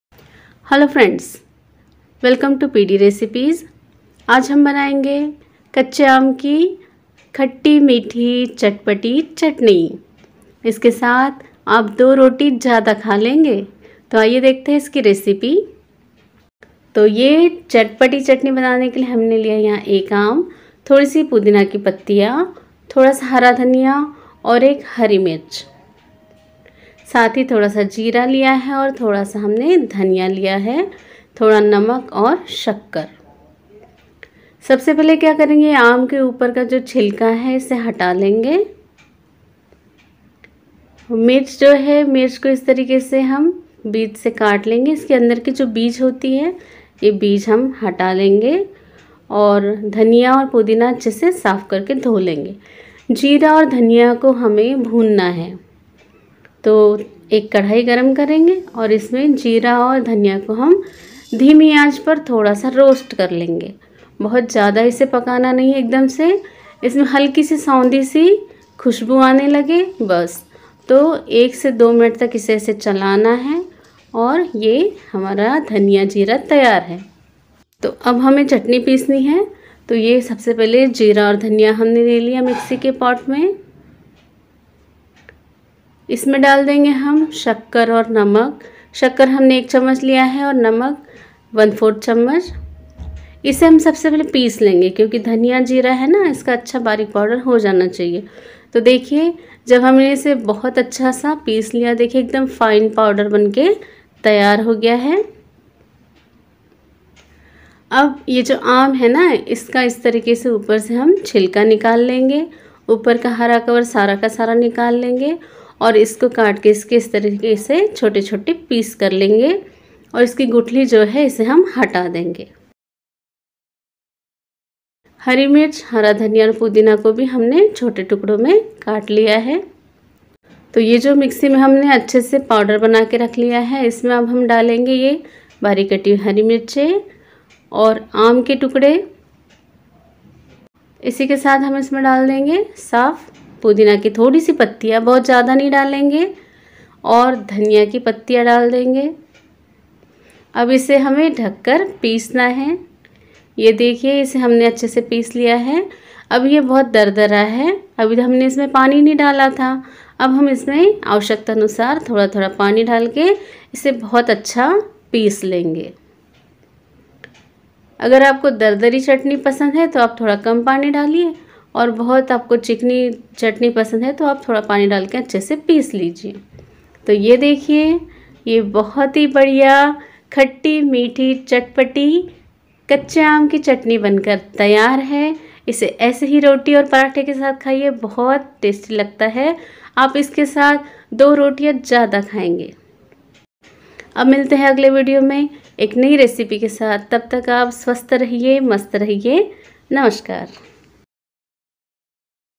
आम की खट्टी मीठी चटनी || Mango chutney by PDrecipes
आम की चटनी खट्टी मीठी आप दो रोटी ज्यादा खाएंगे हाजमा भी ठीक हो जाएगा || PDrecipes

अगर आपको भूख नहीं लगती है तो हमारे बताए तरीके से बनाइए यह खट्टी मीठी आम की चटनी जिसे खाने से आपकी भूख बढ जाएगी और हाजमा भी ठीक हो जाएगा | इसमे है पुदीना, जीरा और धनिया जिससे पेट ठीक हो जाता है और अच्छी भूख लगती है |

Do try this yummy and easy recipe and drop a comment . See you soon .